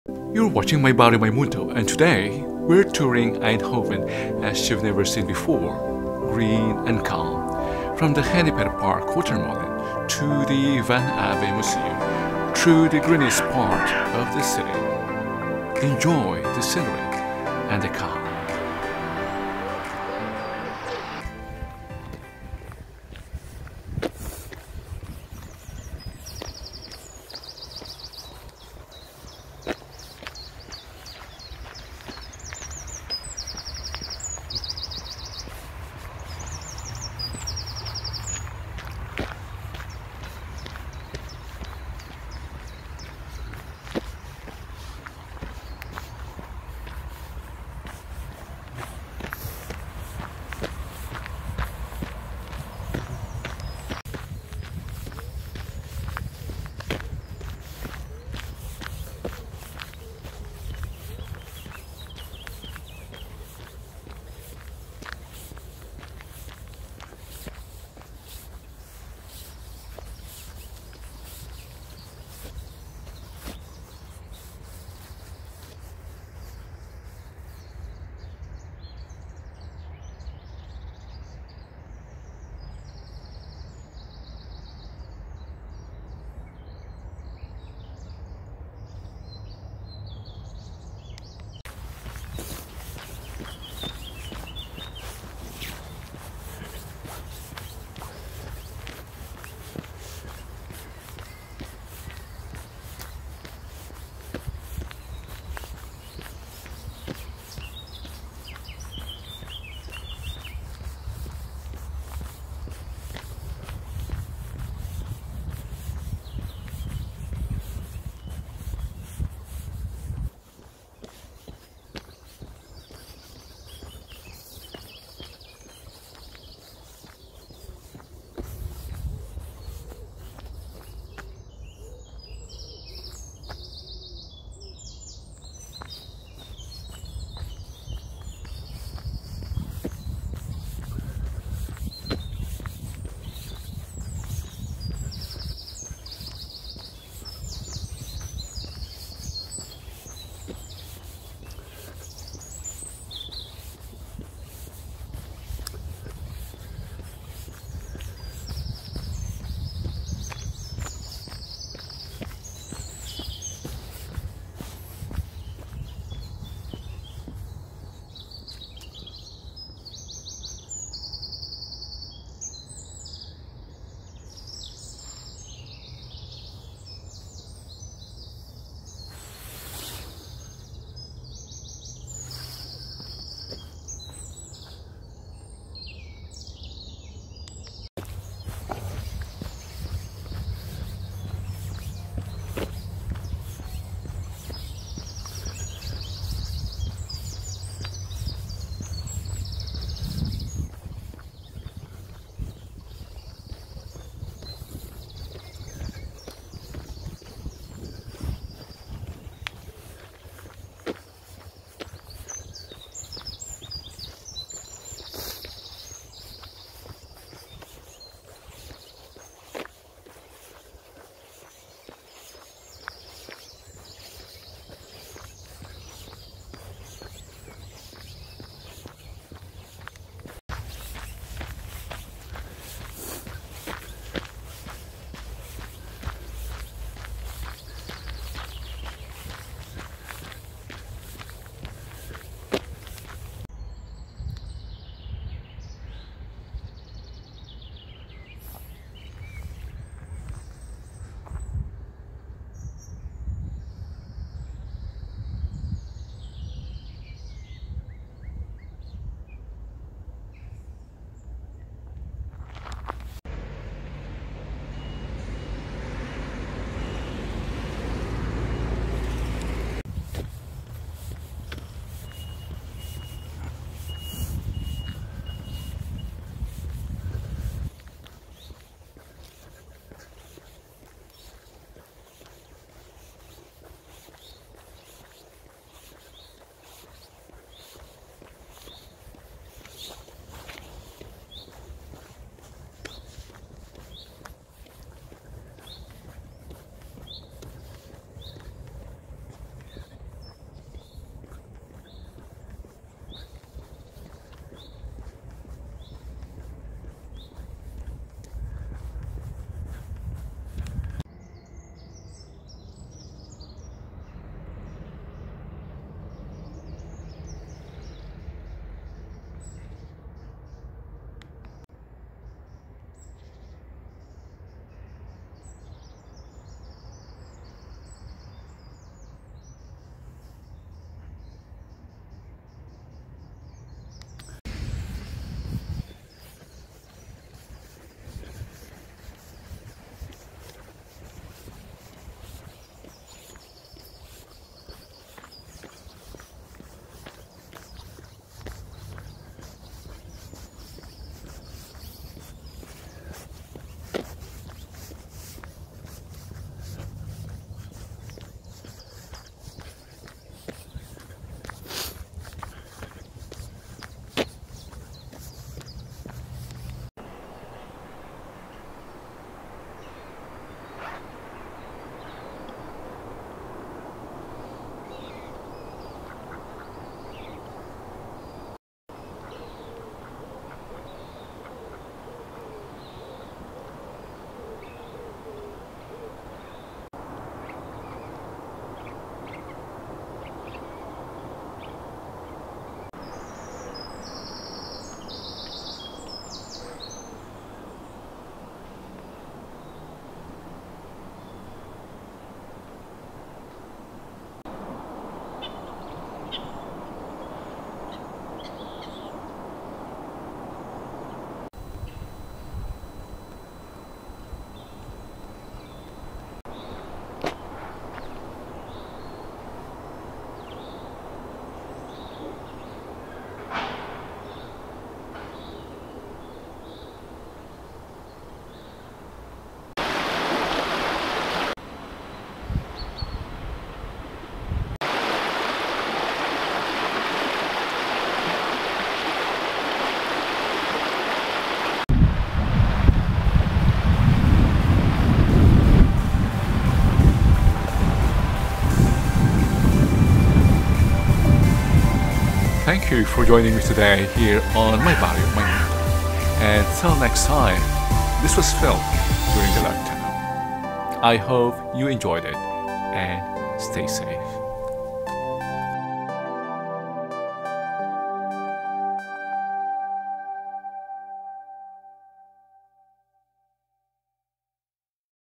EINDHOVEN - Short Tour in the the park - Travel Guide - Netherlands (2020)
Eindhoven is not just high-tech aka Brainport. It is also green. In this walking tour, I will be showing the green areas from Genneper Park to van Abbe Museum. I never thought Eindhoven can be this beautiful and green.
Join me in this short Tour in the the park.